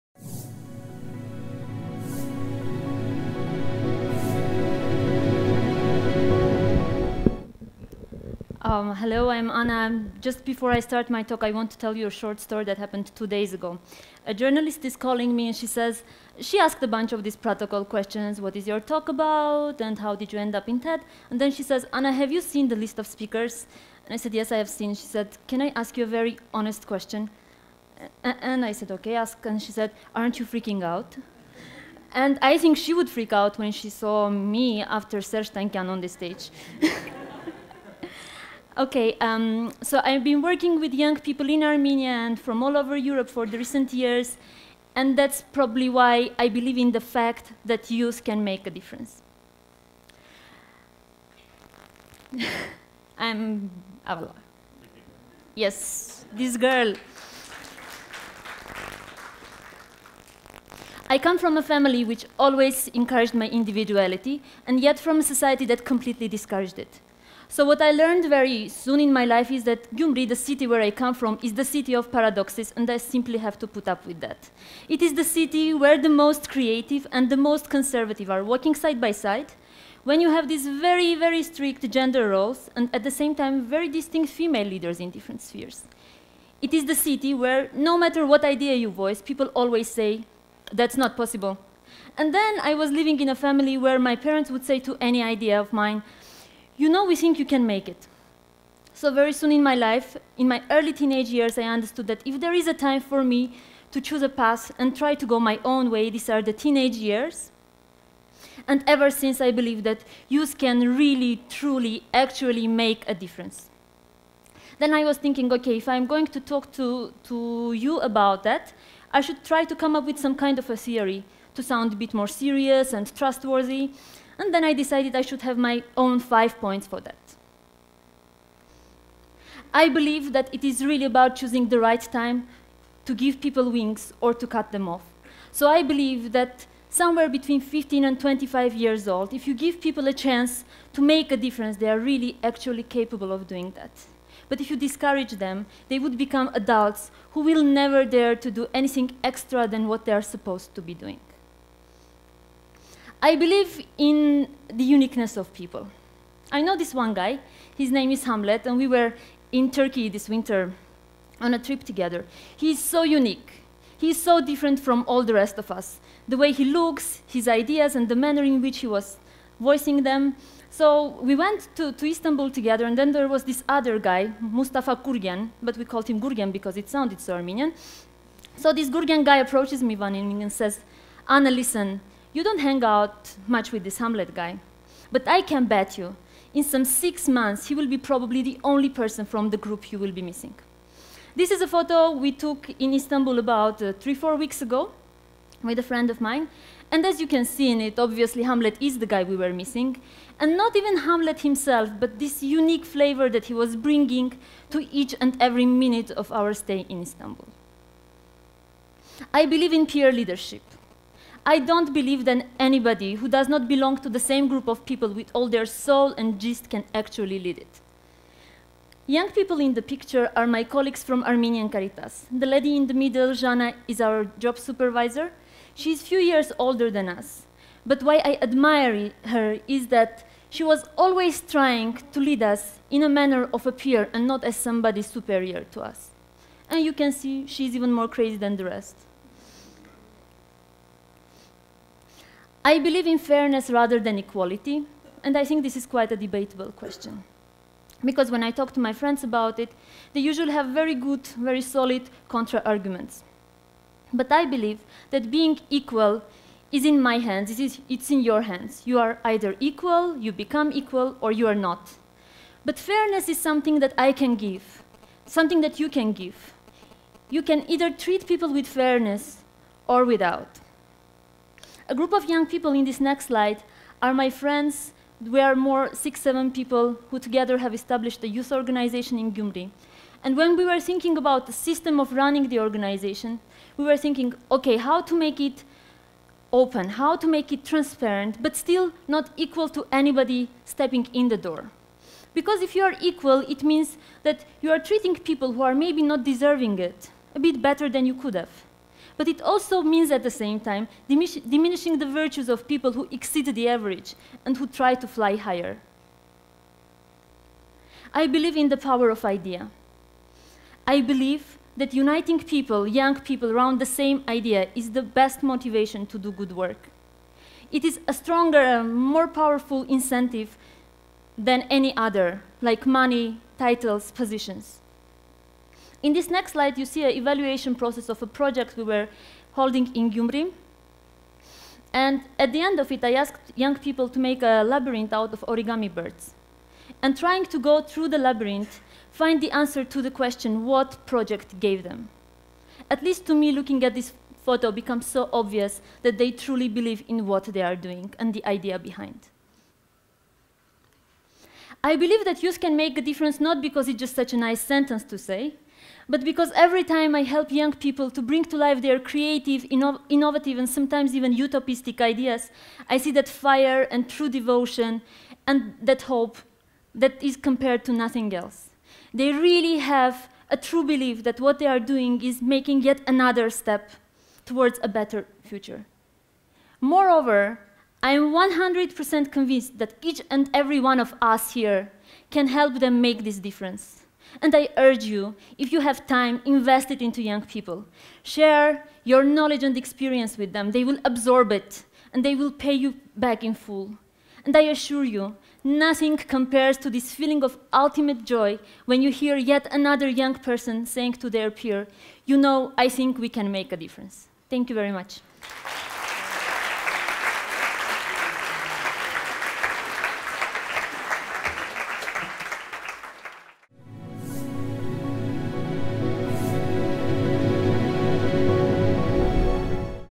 TEDxYerevan - Anna Yeghoyan - YOUth Can Make a Difference
Anna Yeghoyan has been working in the public sector for the past seven years with the focus on youth development through participation and volunteerism. She currently chairs the international board of directors of the Gyumri-based non-governmental organization "Youth Initiative Center", of which she is a co-founder. She is also employed in the community, working on women development projects at Armenian Caritas